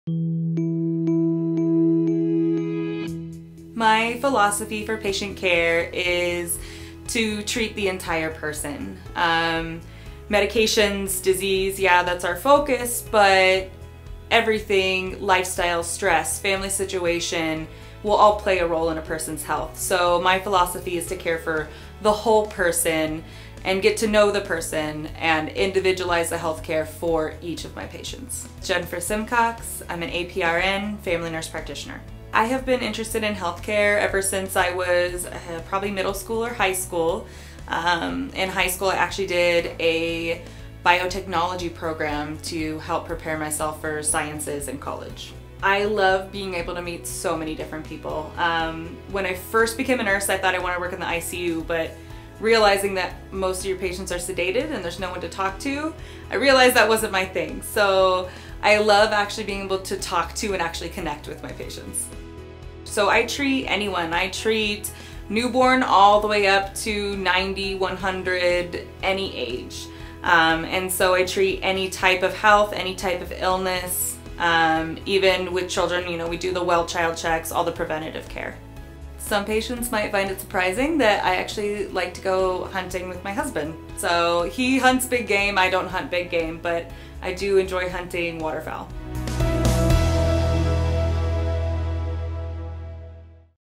Jennifer Simcox, Primary Care Nurse Practitioner
Jennifer Simcox, APRN, believes in treating her patients as a whole, getting to know them, and individualizing the healthcare for each patient. She enjoys meeting so many different people and be able to talk to and connect with her patients. She sees patients from newborns to seniors. In her spare time, she enjoys going water-fowl hunting with her husband.